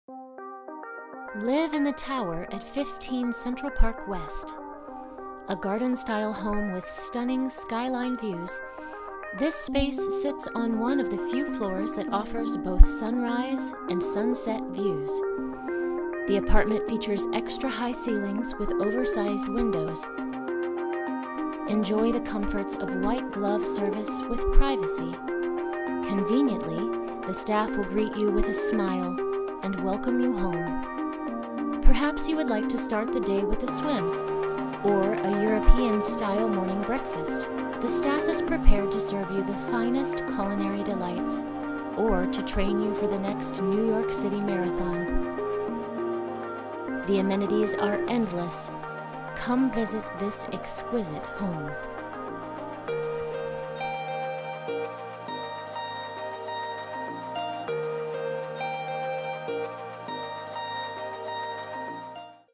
15 Central Park West Apt. 32C, Upper West Side Manhattan, New York
Live in the Tower of 15 Central Park West. If you value a garden style home with stunning skyline views, I invite you to experience this haven. One of the few floors that offers both sunrise and sunset views. Enjoy the comforts of amazing white glove service with privacy. The moment you enter the motor court, you will feel at home. Conveniently, the staff will greet you with a smile and welcome you home. Arriving at 15 Central West will make you feel cared for and relaxed. Simply put, living on Central Park West is like living in a garden with fountains surrounding your arrival. Come up to your home in the sky and hear the sounds of a quiet peaceful retreat. This home features extra high ceilings with over sized windows allowing the morning sun to shine in as the day begins. Perhaps you would like to start the day with a swim or European style morning breakfast? The staff is prepared to serve you the finest culinary delights or to train you for the next New York City Marathon. The amenities are endless, simply elevate down to the Spa level and enjoy the health facility. I look forward to showing you this exquisite home. Come home!